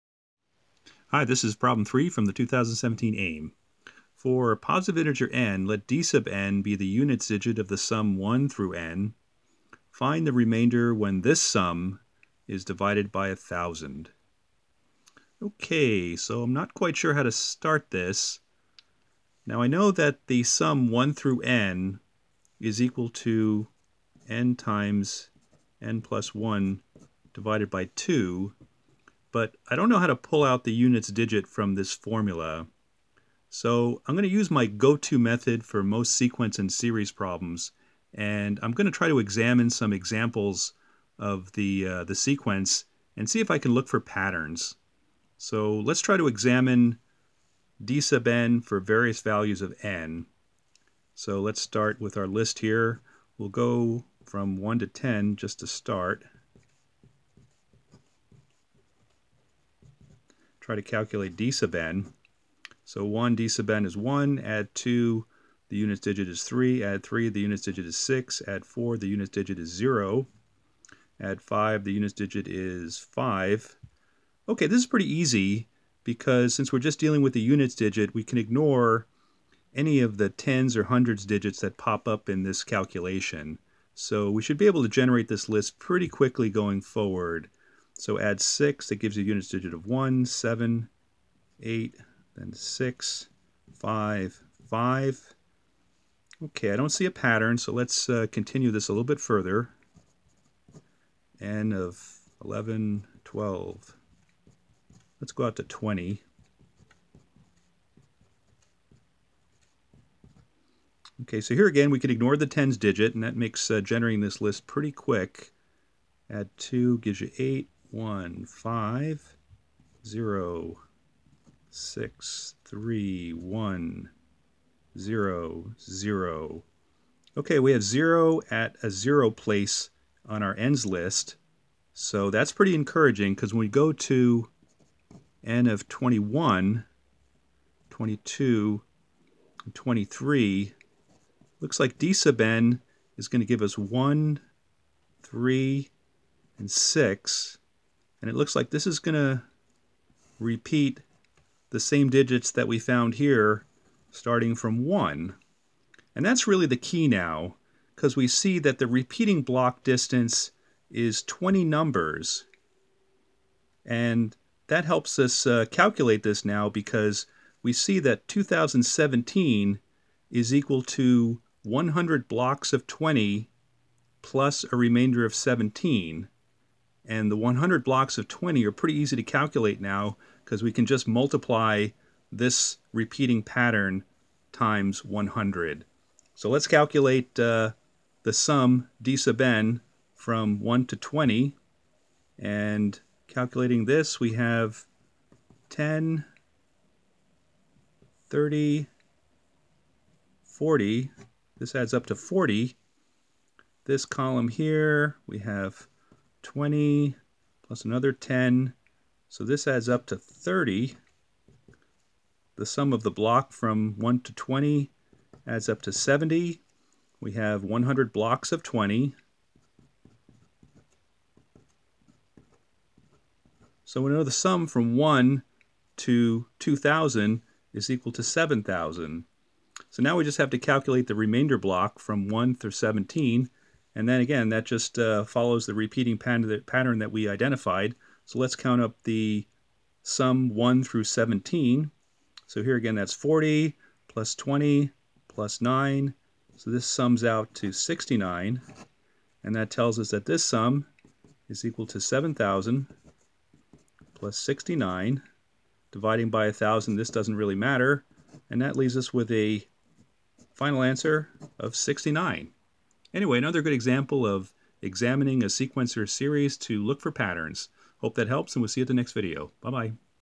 2017 AIME Prob 3
💛 Analyzing a sequence or series to look for a repeating block distance is often helpful in AMC and AIME exam problems.  The repeating block distance of a sequence is called the "period" of the sequence.

🤔 Actually, you can examine n(n+1)/2 (mod 10) to discover the dn sequence first repeats when n(n+1) is a multiple of 20.  You still need to bash the numbers to find the sum d1+d2+...+d20.